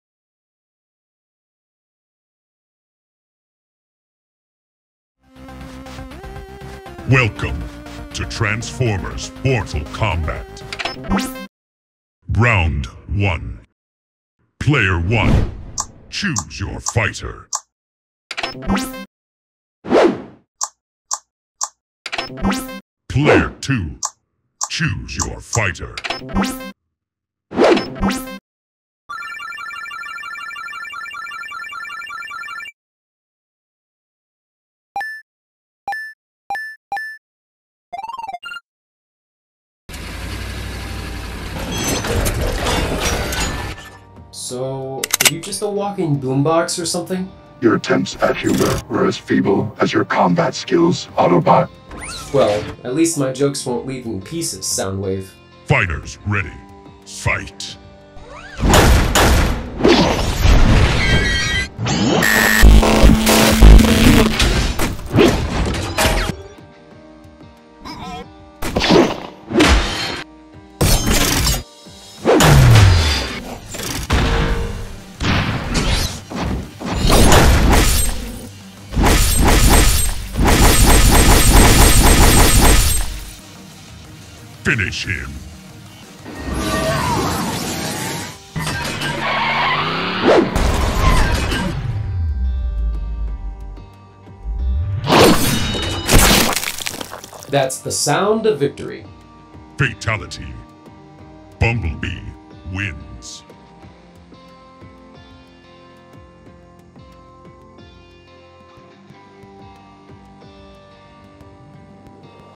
Transformers Mortal Kombat Round 1: Bumblebee vs Soundwave
Here we go my gang!!! Today is the the first episode of my new short series!!

This is more of a trial run to be honest but I’m really happy with how it came out! Plus, episode two is one that I think you guys will enjoy ;)

Until then, enjoy the pilot episode!!!